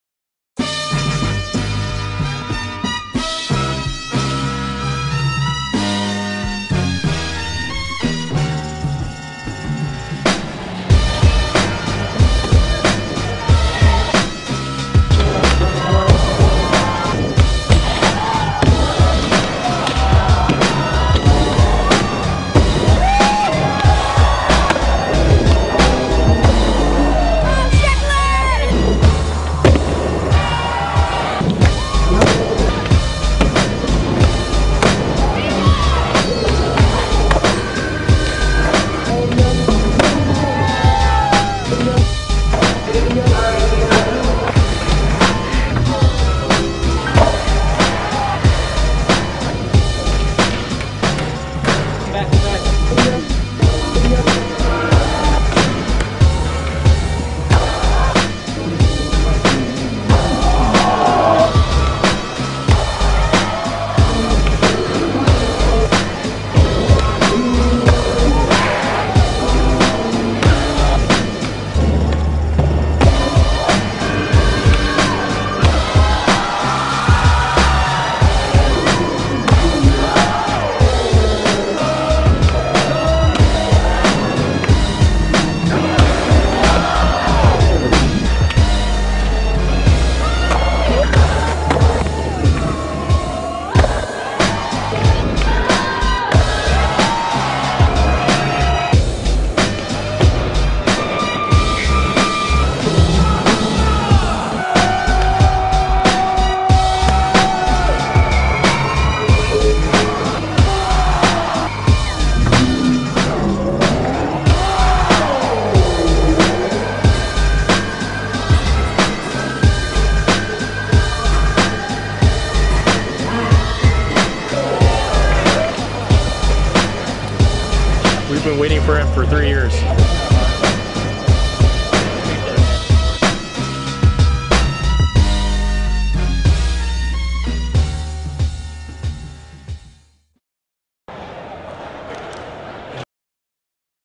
Plan B United Tour Vans Skatepark
the plan B skate team demos at vans skatepark